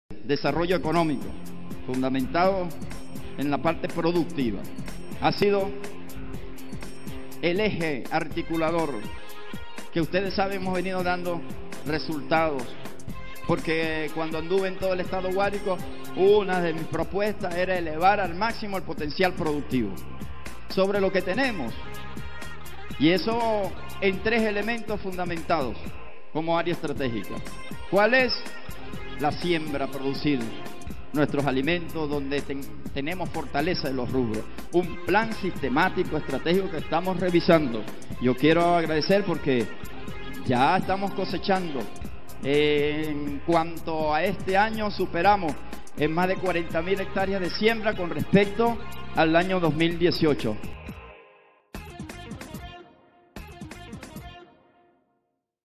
ÁREA PRODUCTIVA EN LOS 2 AÑOS DE GESTIÓN DEL GOBERNADOR JOSÉ VÁSQUEZ
Llevando la producción estadal y Nacional como objetivo primordial en su gestión el Gobernador José Vásquez se ha encargado de potenciar esta línea estratégica.